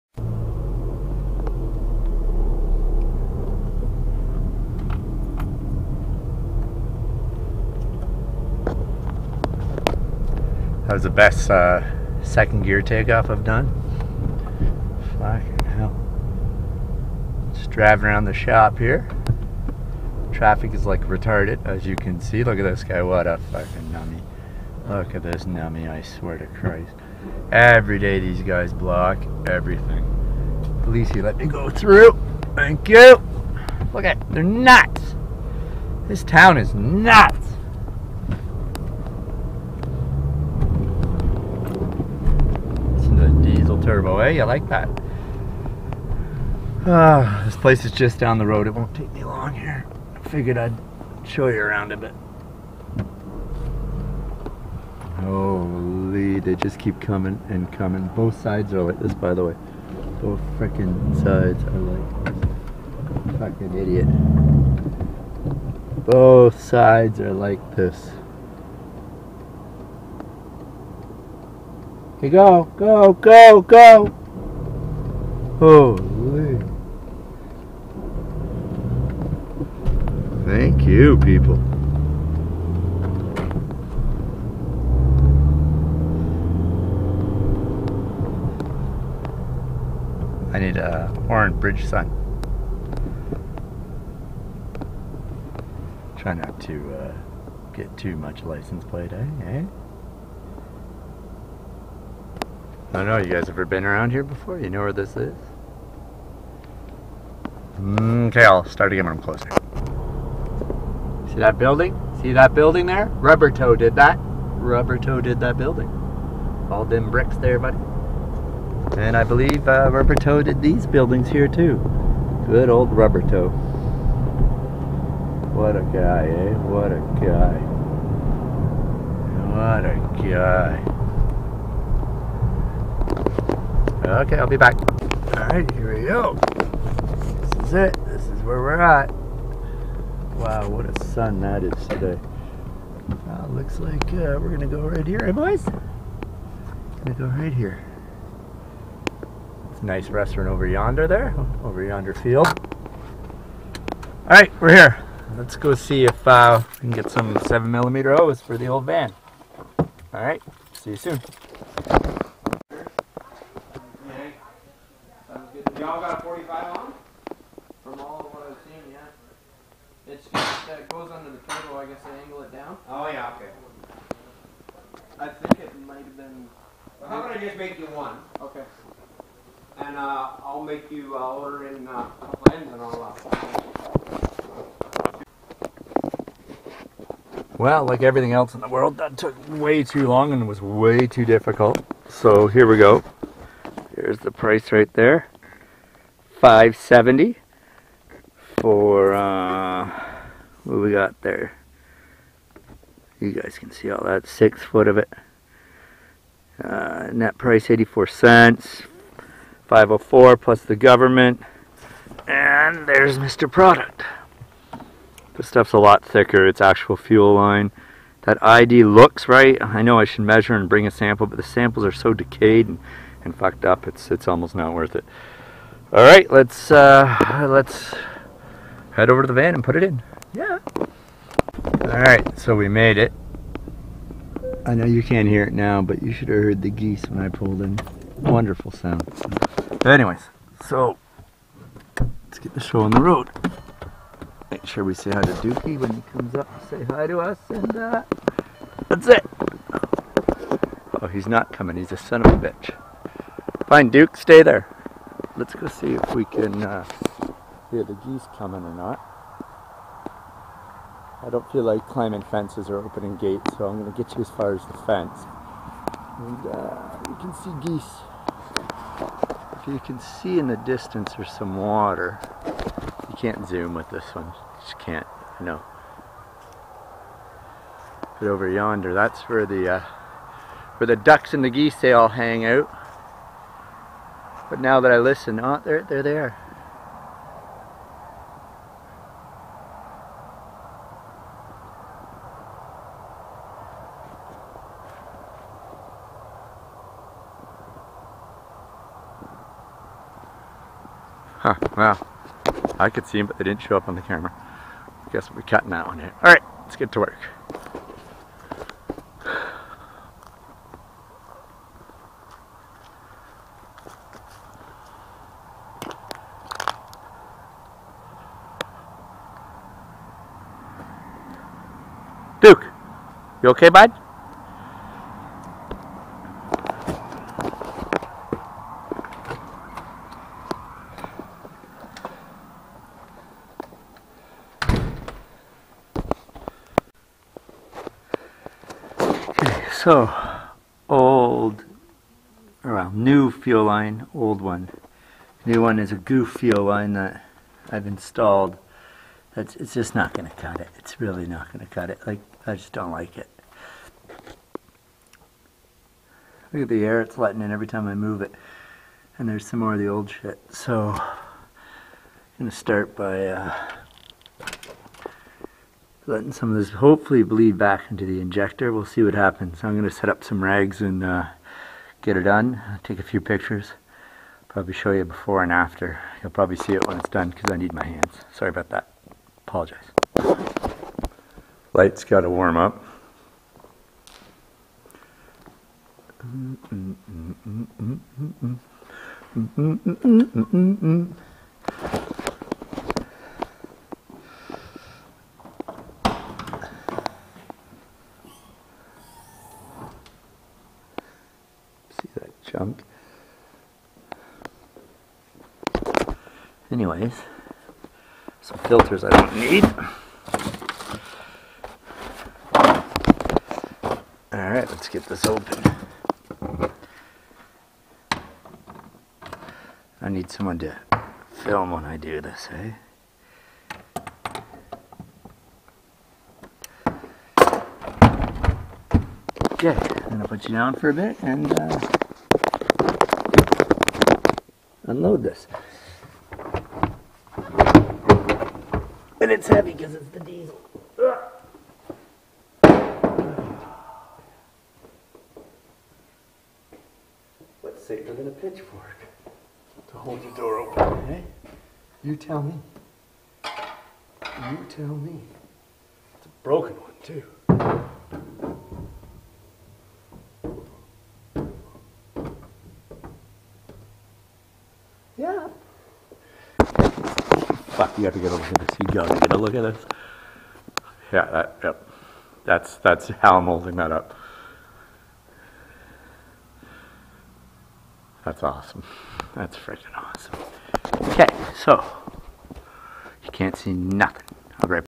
Vanagon Repair
looking to replace fuel line on the Vanagon twitter @Artarous